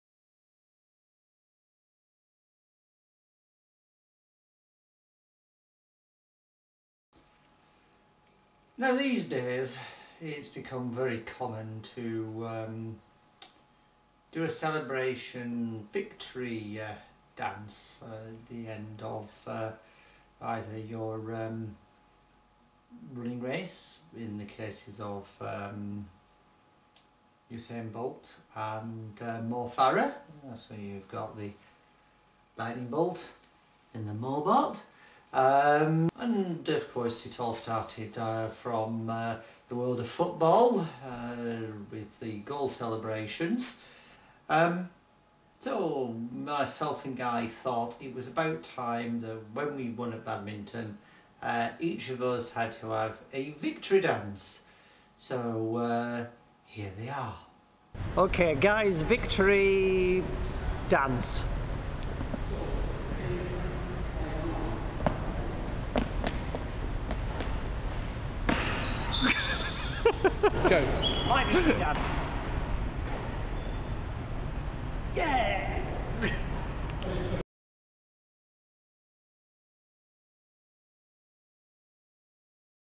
23 August 2012 - VB277 - Victory Dances
Video Blog 277 - As all top sports people have a victory dance myself and Guy decided it was time we should also have dances for when we won at Badminton.

The football pictures used in the opening segment of the video are by kind permission of